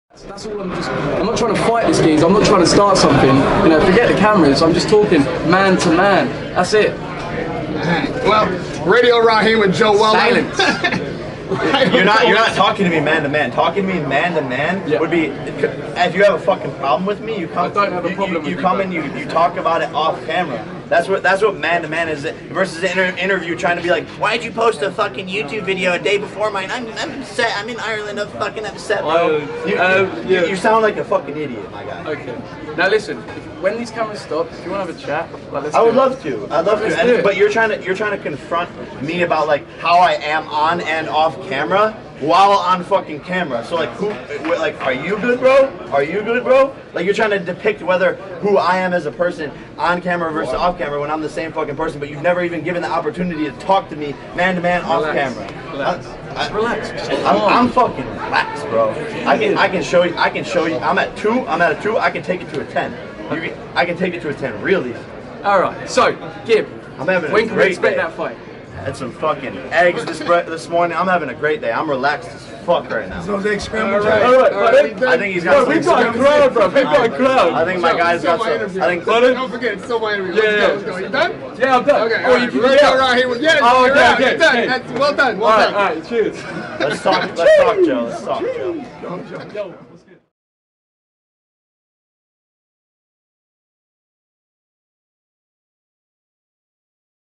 JAKE PAUL & JOE WELLER CONFRONTATION; TRADE WORDS "AT A 2" BEFORE TAKING IT TO A 10
FightHype.com was on hand in Los Angeles, California where YouTube stars KSI and Logan Paul held the final press conference leading up to their pro debut for their highly-anticipated rematch. You don't want to miss what those in attendance had to say. Check it out!